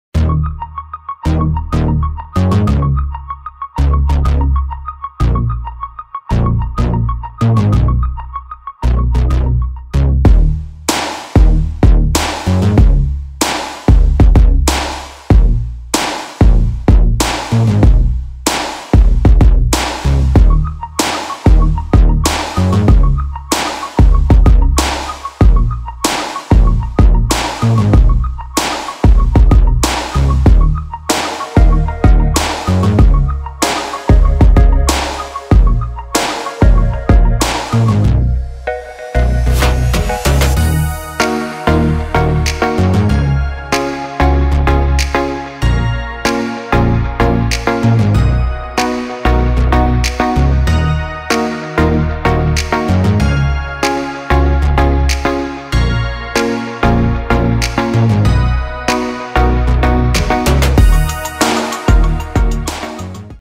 K&F Concept tripod short Ad.
The guys from K&F sent me this awesome tripod and i film a little b-roll ad. The shots that you've seen are shot on the sony a7iii with the 28-70 kit lens.